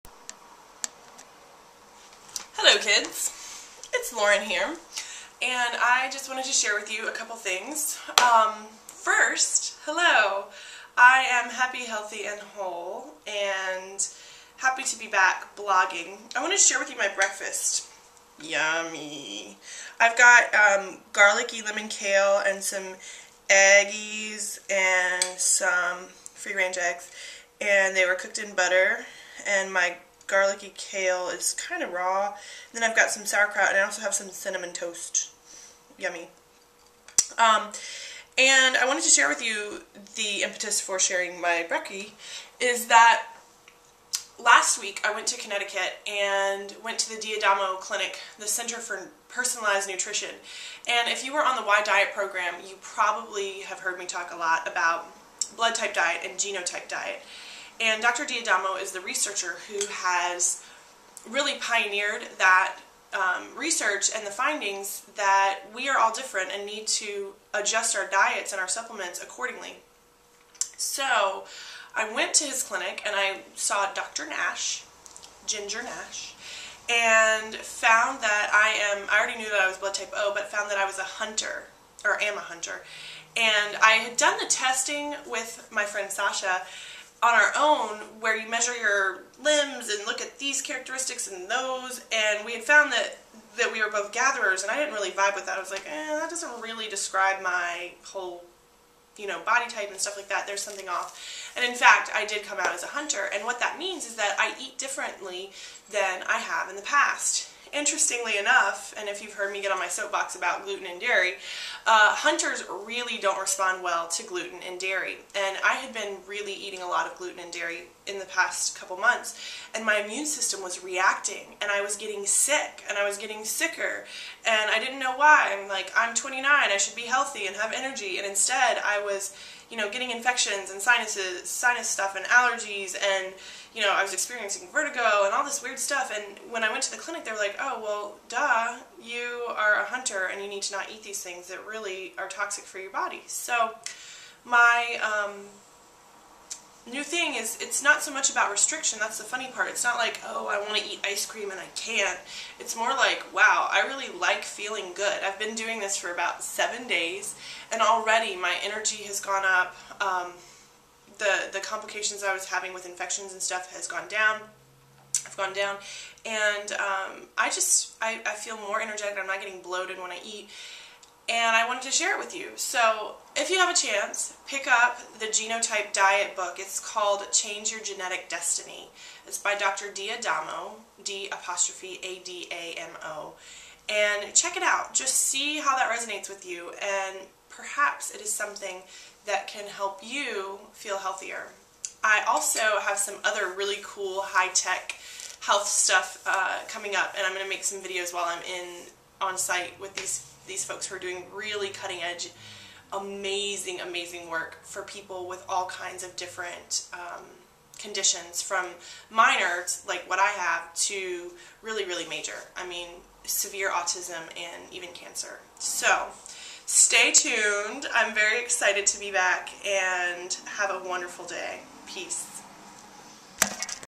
I'm a HUNTER. What are YOU?
So, I've encountered a new way to address wellness...it's called the Genotype Diet. If you were on the "Why Diet" calls, then you've certainly heard me talk about the blood type and genotype concepts. And I found them really compelling so I went to the clinic in Connecticut to have my genotype assessed.
The results?

I'm a hunter! And that means that I need to eat and exercise a specific way to be healthy.

After only 7 days following the Hunter recommendations, I feel better, digest my food better, am making better food choices, and have more energy. This is fantastic!